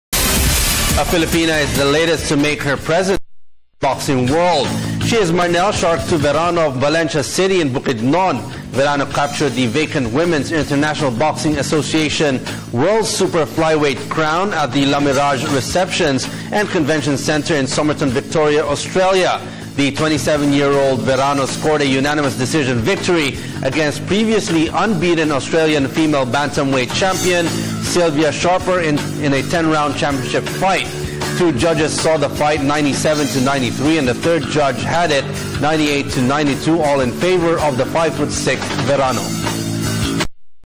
NewsLife: Marnelle Verano wins boxing world title in Australia || May 11, 2015
NewsLife: Marnelle Verano wins boxing world title in Australia || May 11, 2015 (Reported by Robert Tan)